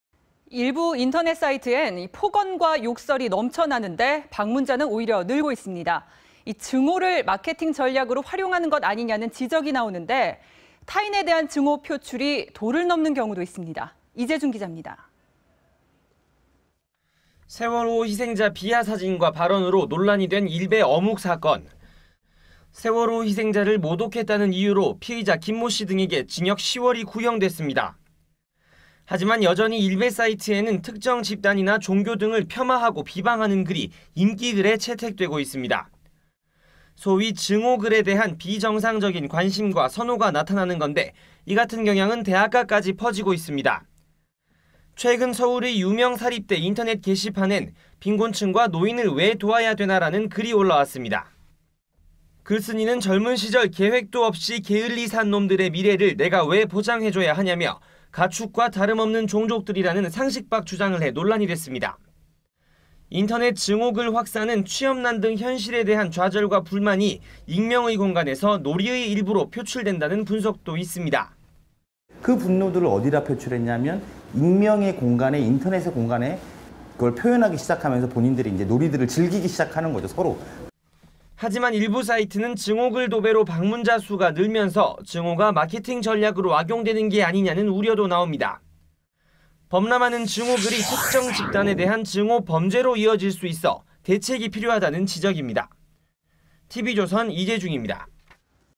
'증오가 놀이로'…일베·유명 대학 커뮤니티 까지
일부 인터넷 사이트엔 폭언과 욕설이 넘쳐나는데 방문자는 오히려 늘어 증오를 마케팅 전략으로 활용하는 것 아니냐는 지적이 나옵니다. 타인에 대한 증오 표출이 도를 넘는 경우도 있습니다.